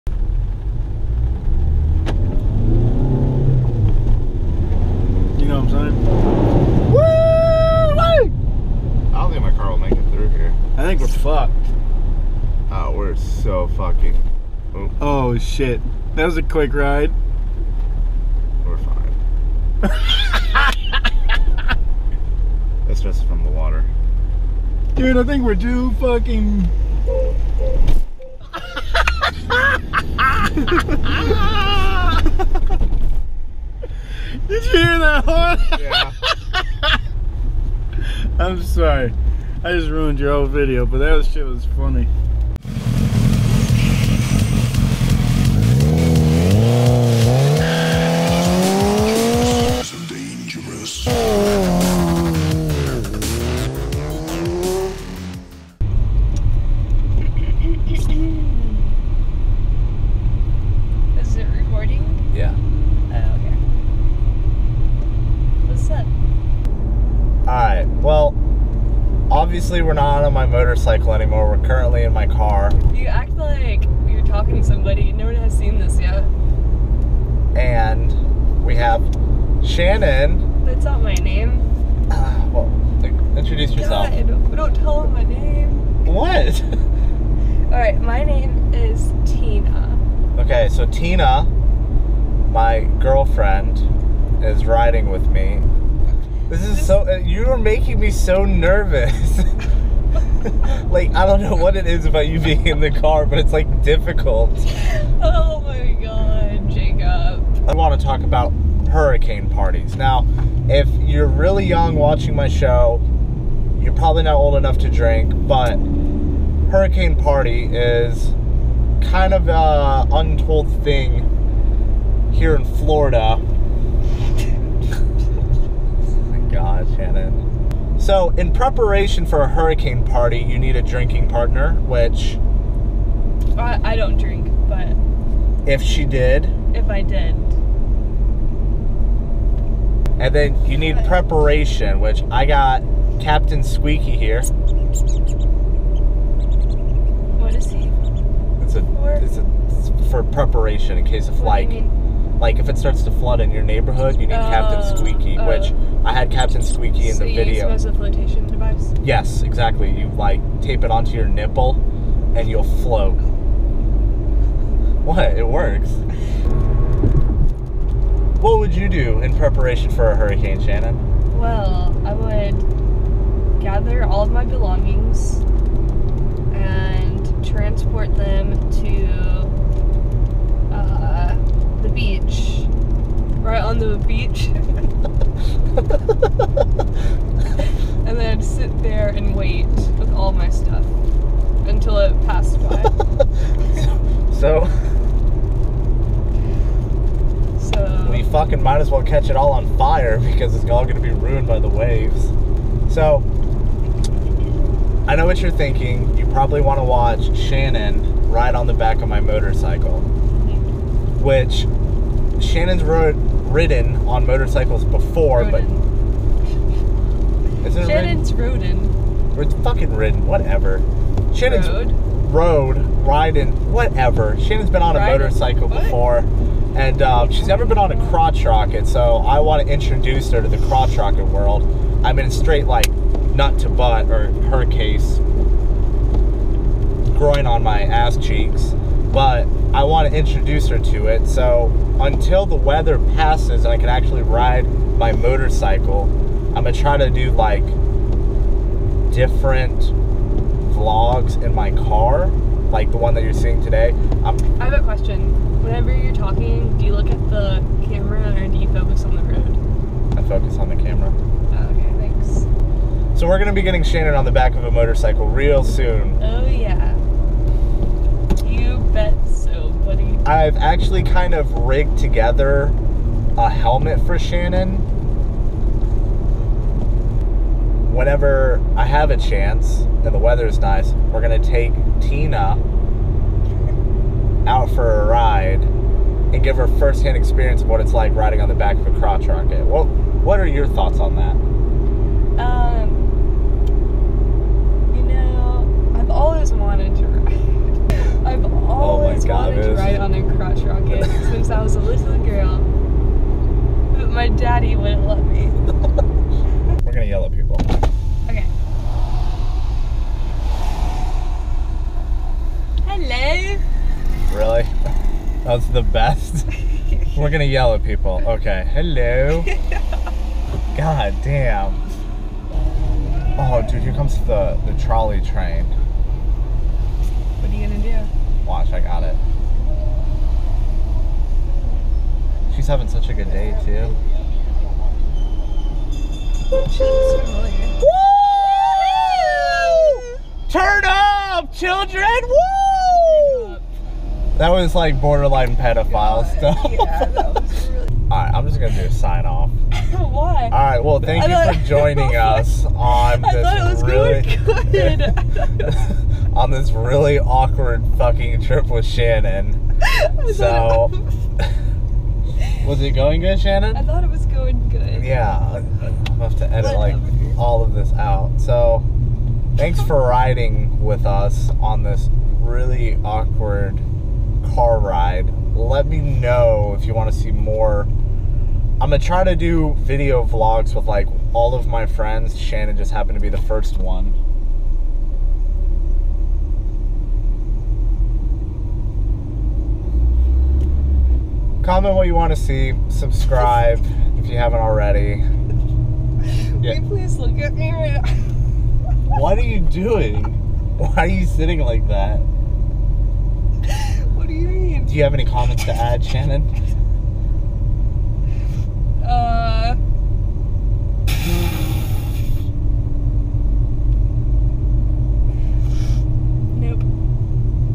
Cruising with style! feat. Shannon/"Tina"
So with the weather being as depressing as my car, I decided to take it upon myself to invite Shannon/Tina on a nice car ride with me!